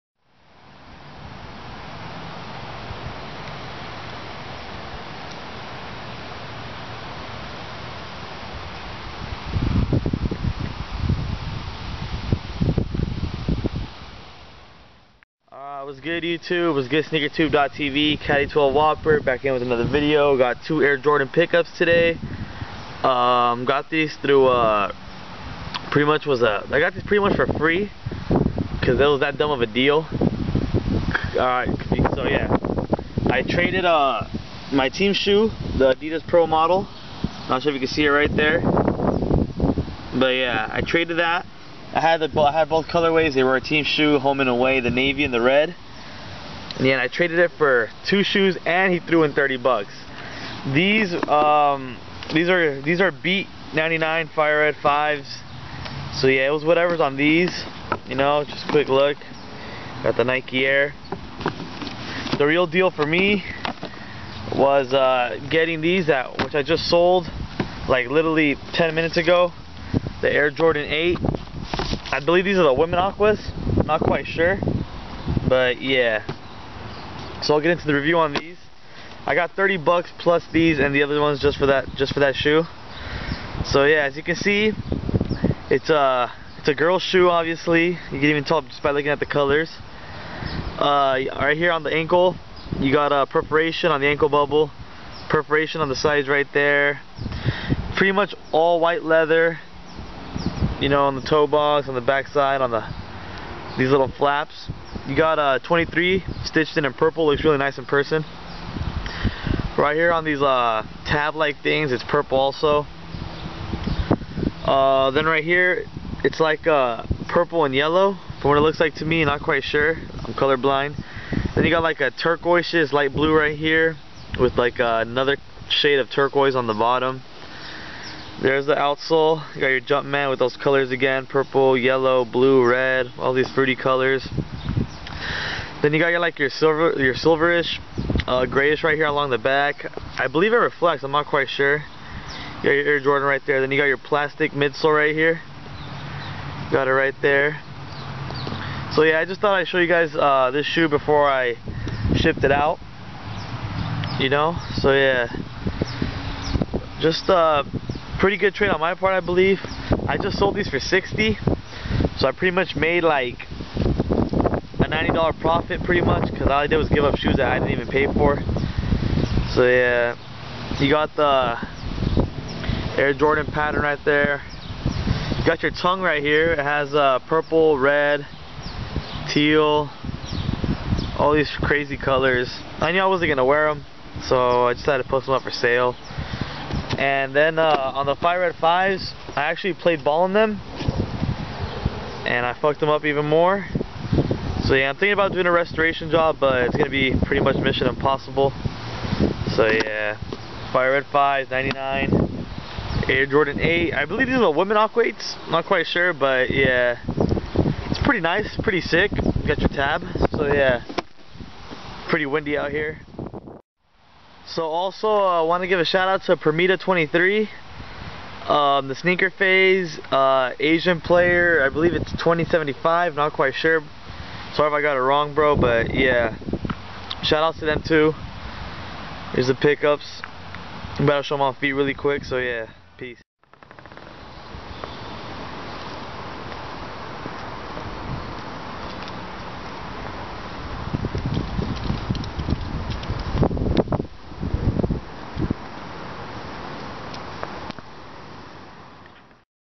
Air Jordan #8 #9 '99 Fire red v's & Aqua 8s (women)
traded two beat adidas i got for free for these two shoe splus 30$ for me whatta deal and i just sold the 8's for 60 :D